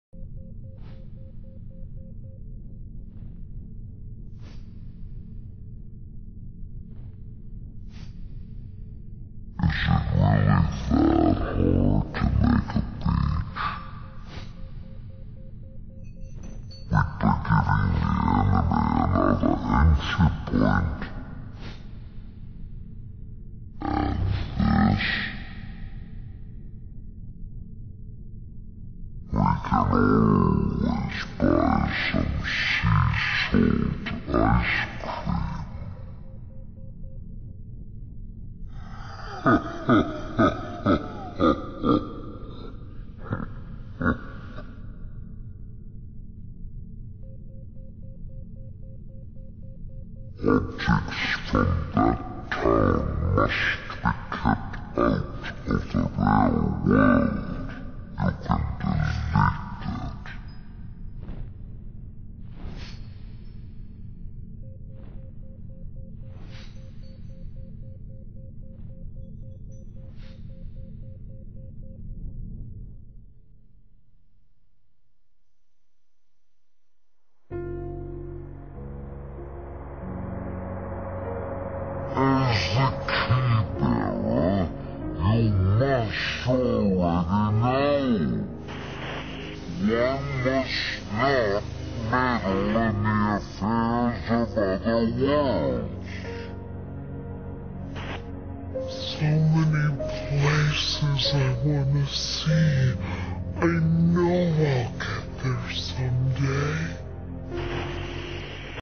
Slow motion super funny
What happens when my friends whant me to slow down a scene. DiZ Sounds evil when he laughs. :P